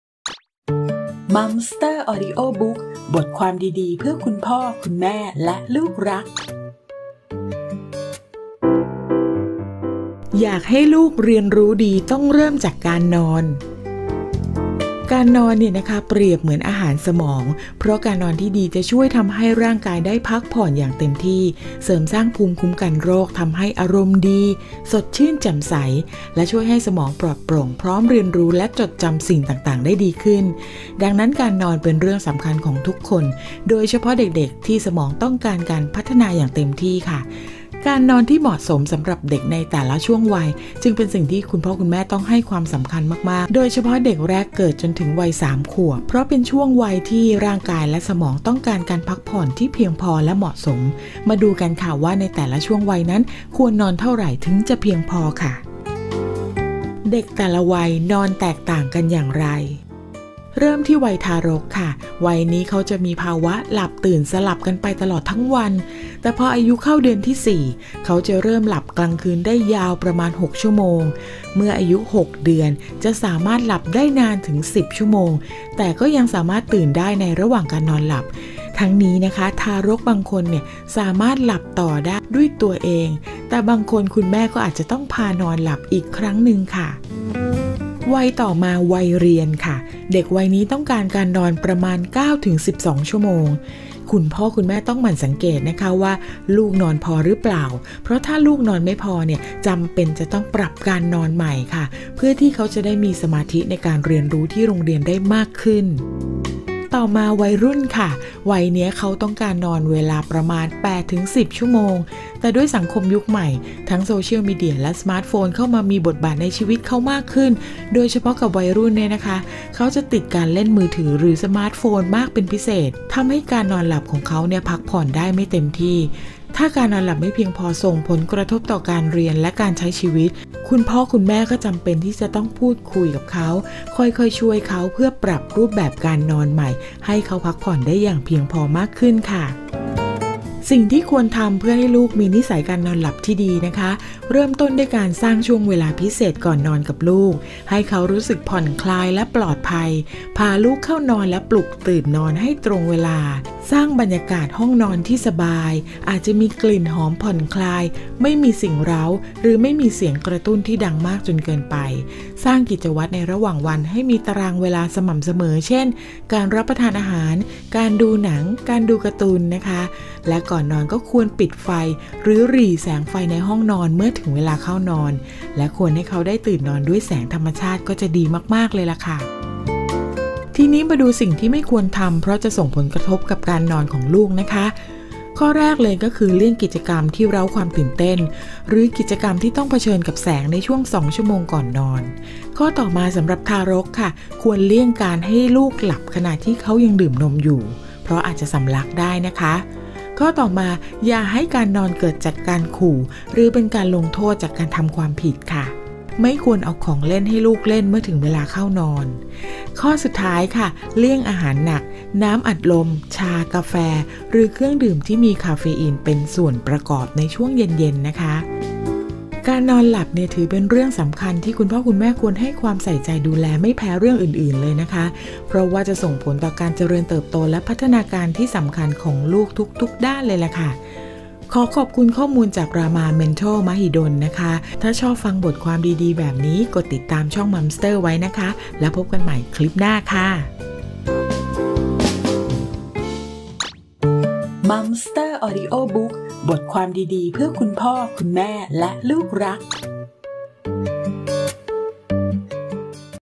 อยากให้ลูกเรียนรู้ดี เริ่มจากการนอน | Momster Audiobook
การนอนของเด็ก ในแต่ละช่วงวัยมีความสำคัญต่อการเจริญเติบโต และพัฒนาการในด้านต่างๆ การเรียนรู้ที่ดี เริ่มต้นจากการที่ร่างกายได้รับการพักผ่อนที่เพียงพอค่ะ

คุณพ่อคุณแม่ควรเข้าใจธรรมชาติการนอนของลูก ในแต่ละช่วงวัย เพื่อดูแลลูกได้อย่างเหมาะสม คลิปนี้มีข้อมูลดีๆ มาฝากค่ะ